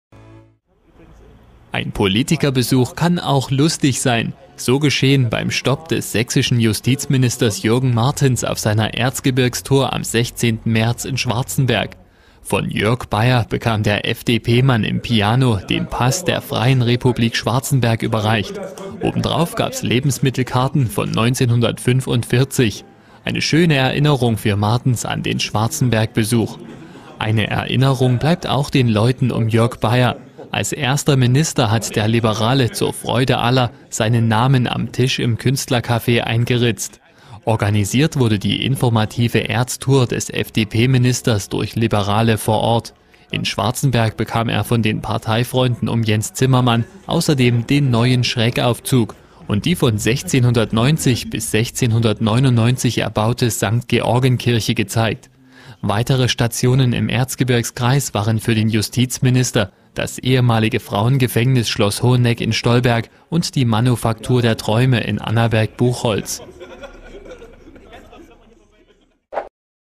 Staatsminister Dr. Jürgen Martens, FDP, in Schwarzenberg Erzgebirge
Eine „gute Bewährung" vorausgesetzt, hat Sachsens Justizminister Jürgen Martensbei einer Stippvisite in Schwarzenberg den Einreisepass für den Aufenthalt in der „Freien Republik" erhalten. Ausgehändigt wurde ihm das Dokument nach Befragung und Belehrung durch Jörg Beier, den „Präsidenten der Freien Republik", nachdem das Dokument ausgestellt wurde. Der Besuch im Künstlercafé „Piano" war jedoch nur eine kurze, humorvolle Station auf der Besuchsreise des Politikers durch das Erzgebirge. Diese führte ihn von Stollberg über Schwarzenberg nach Annaberg- Buchholz und weiter nach Olbernhau und Seiffen. In Schwarzenberg zeigte sich Jürgen Martens vor allem vom neuen Schrägaufzug am Schloss begeistert.
Dessen Benutzung hatte sein Parteifreund Jens Zimmermann mit in die Tour
eingebaut. „Das ist eine tolle Sache, eine gelungene Idee für die Menschen, und
sie wurde super umgesetzt", lobte Martens die Neuheit am Wahrzeichen der Stadt. Für ebenso originell hält er die Idee der Künstlergruppe, die Pässe mit
Lebensmittel-Karte von einst auszureichen.